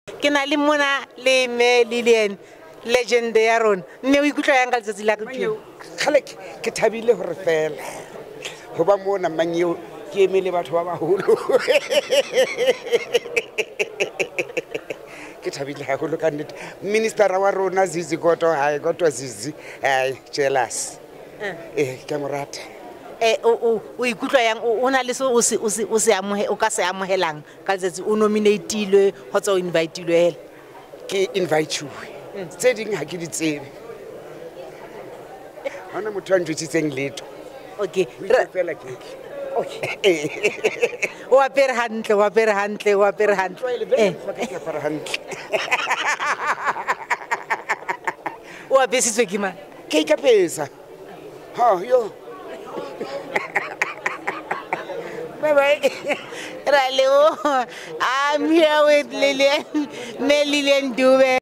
Shocking Recognition of Lillian Dube at Prestigious
Celebrating Southern African Culture At The Cultural And Creative Industry Awards! 🌍✨ Let's Honor Our Rich Heritage!

More news, visit mdntvlive.com and Mdntv on all Social Media platforms.